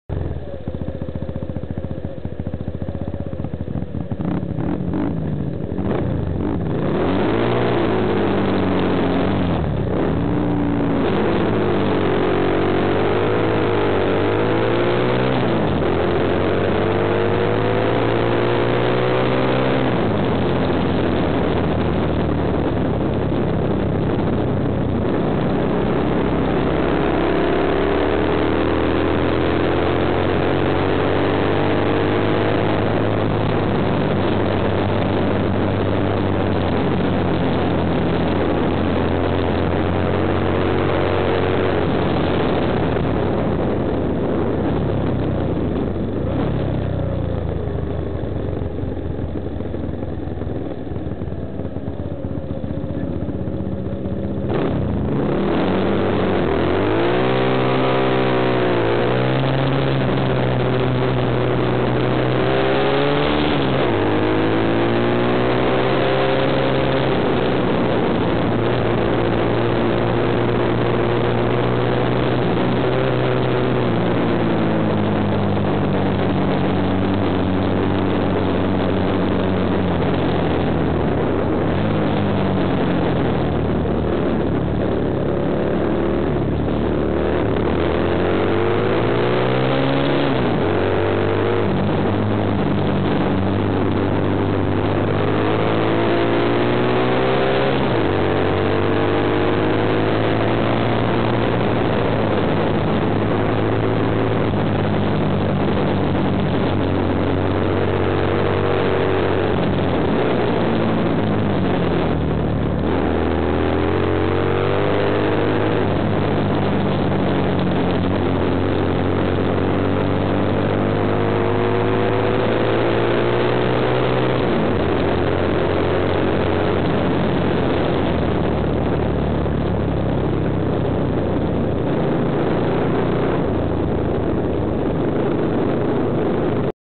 キャピタルスポーツランド 練習４.avi
キャピタルスポーツランドにてKSR110の車載カメラから撮影